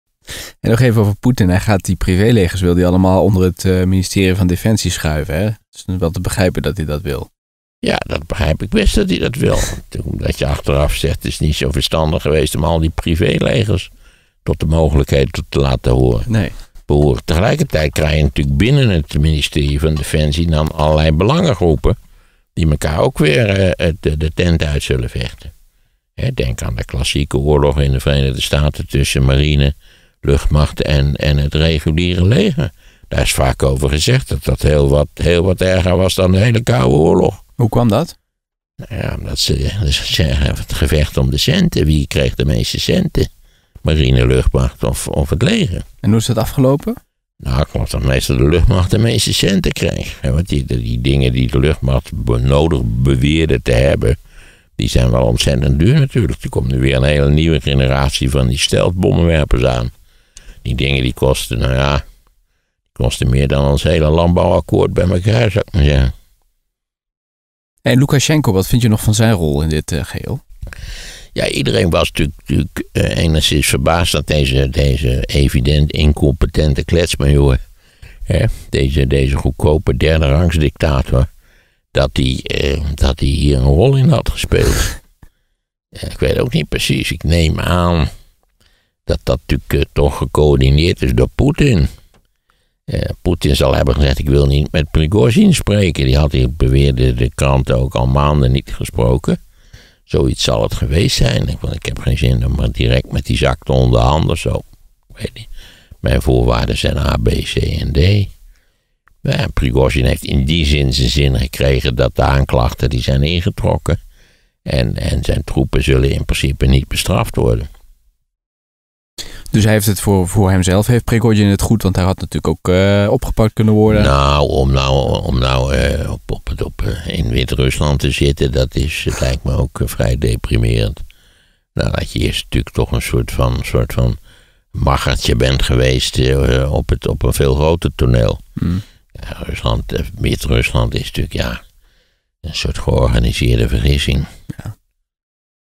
Waar is Jevgeni Prigozjin?
Waar is de leider van de Wagner Groep: Leider Jevgeni Prigozjin?

▬ SHOWNOTES  ▬▬▬▬▬▬▬▬▬▬

▬ MEER MAARTEN  ▬▬▬▬▬▬▬▬▬▬

▬ LUISTERBOEKEN  ▬▬▬▬▬▬▬▬▬▬

▬ SOCIALS  ▬▬▬▬▬▬▬▬▬▬